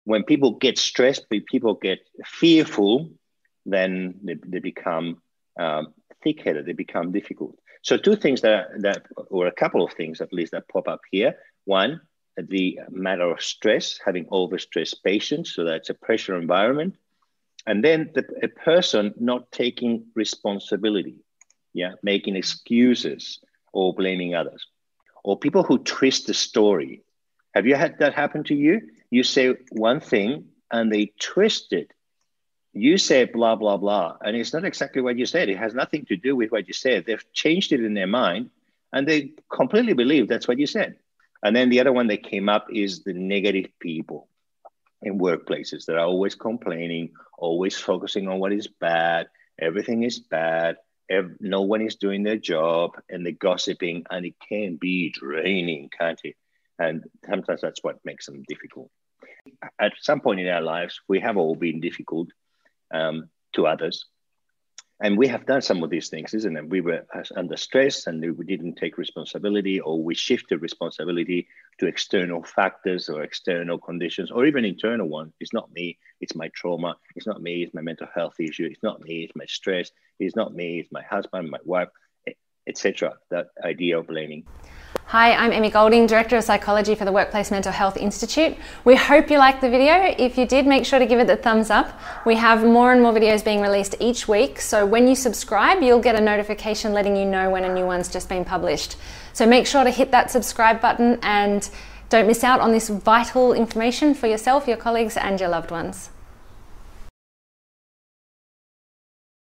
What Happens In The Psychology Of Nice People When They Suddenly Become Difficult People
What happens in the psychology of Nice People when they Suddenly Become Difficult People? How do we deal with difficult people? Nice people can become difficult people for many reasons. If you've ever had an encounter with a difficult person, it's easy to judge and to think that this difficult person is a bad person. It may be a person that continuously makes excuses or blames others. Or you might encounter someone who twists stories or people that are always complaining. Or maybe it's you? ever doubted yourself and thought you might be bad?

In this short video, Peter Diaz and Emi Golding from the Workplace Mental Health Institute, explore the psychology of this relevant and timely topic.

📸 Photo credit: Hello I'm Nik on Unsplash